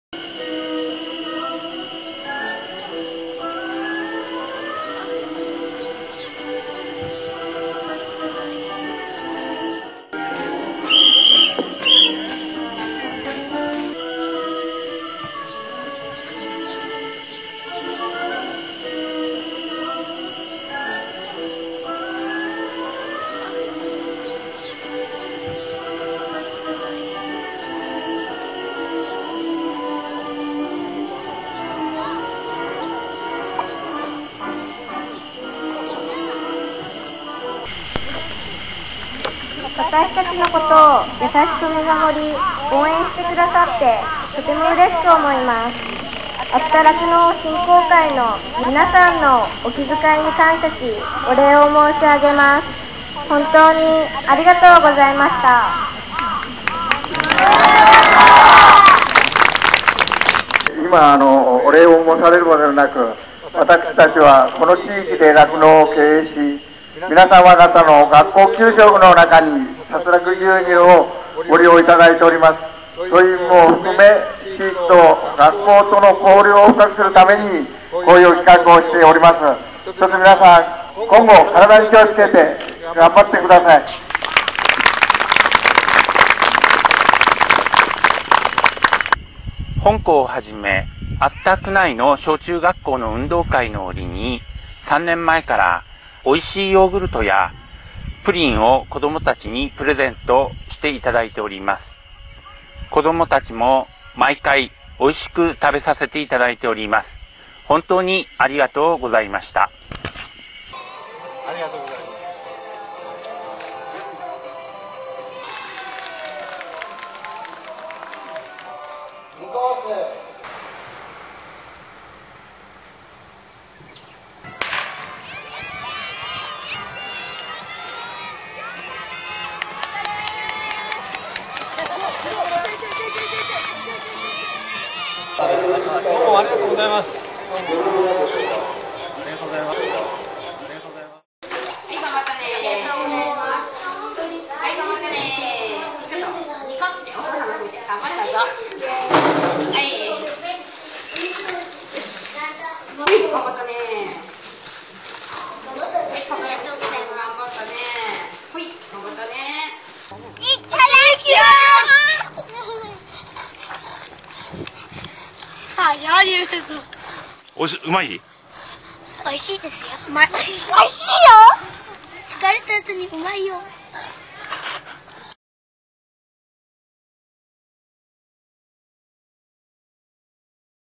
今年も嬉しいプレゼント 2010/07/01(3分00秒)
えりす厚田協力会制作　厚田酪農振興会が乳製品　３年目となる今年も、サツラク農協と共同で運動会当日厚田区の全児童生徒にヨーグルトなどのプレゼントがあたりました